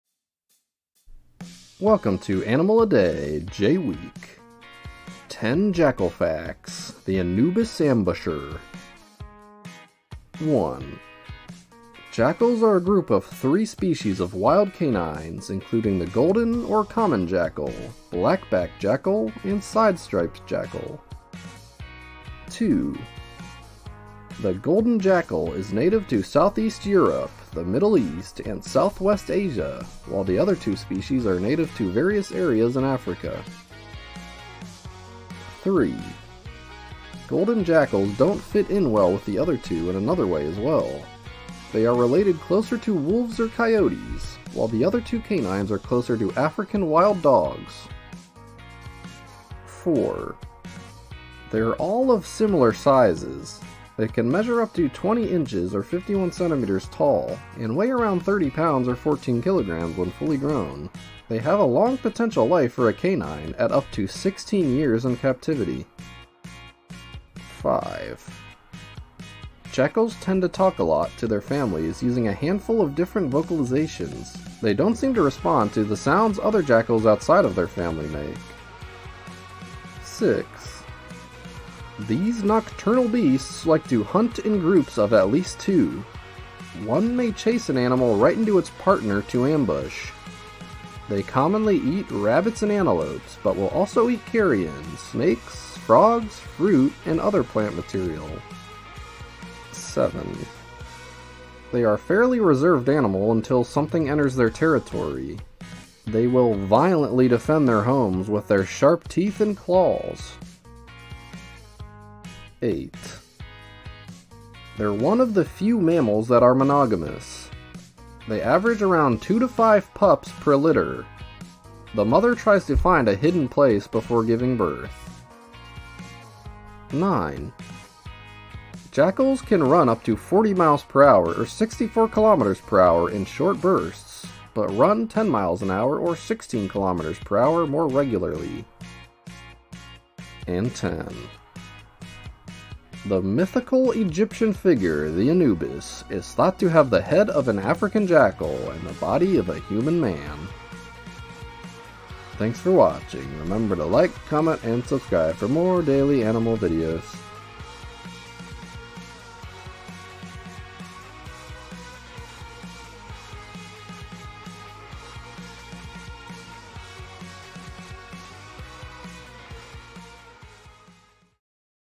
10 Jackal Facts - The Anubis Ambusher - Animal a Day J Week
Welcome to Animal a Day's J Week. Every day this week will have an animal that starts with the letter J. Today we learn about the wild African canine that was the inspiration for the Anubis, the Jackal.